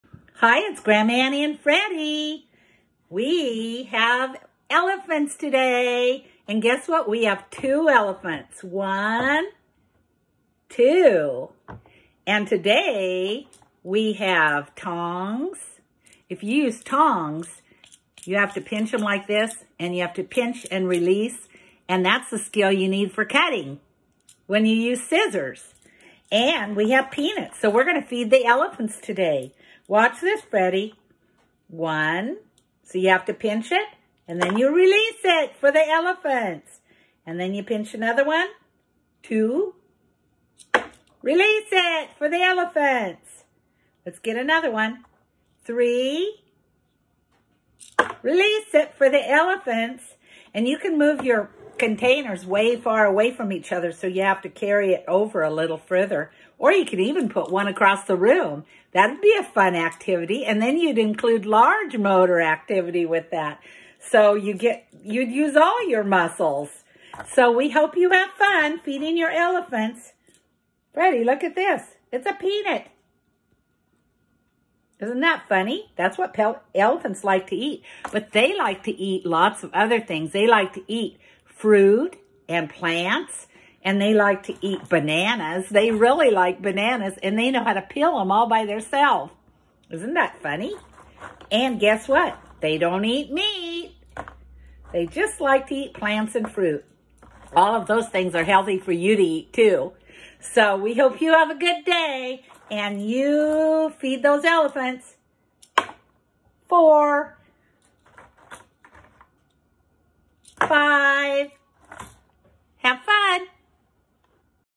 LETS FEED THE ELEPHANTS
Grandma Annie and Freddy use tongs to feed the elephants in a fun activity that will help in developing the skills needed to use scissors.